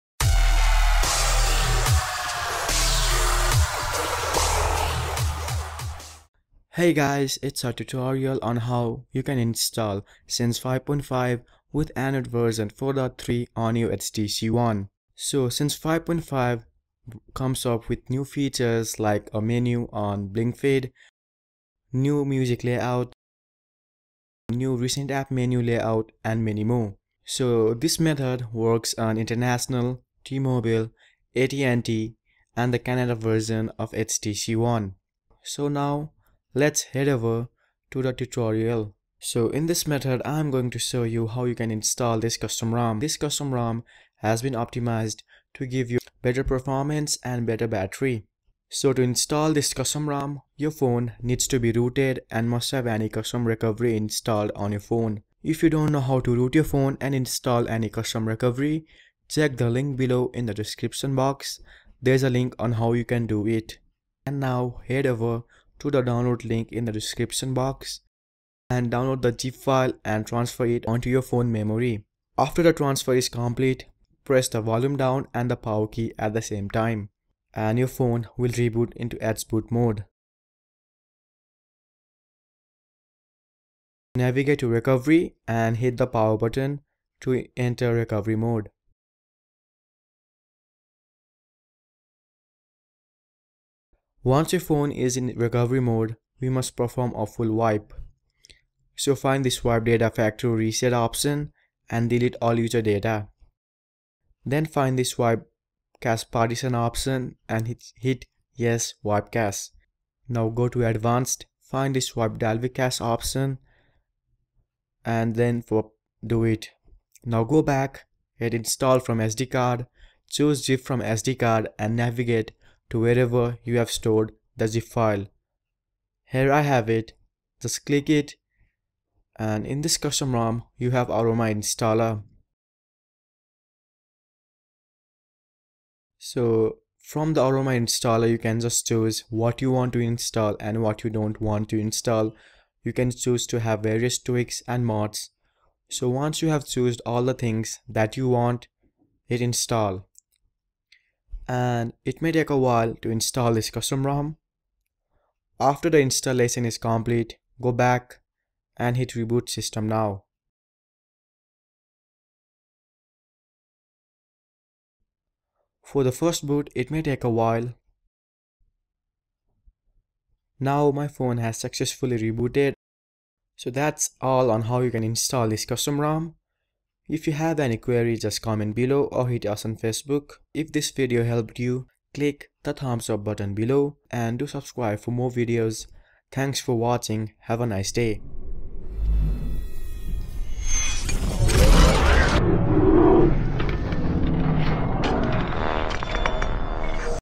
How to Install Sense 5.5 (Android 4.3) on HTC One
its a turorial video on how to install android 4.3 with sense 5.5 on htc one
it works on international , T-Mobile & At&t  HTC One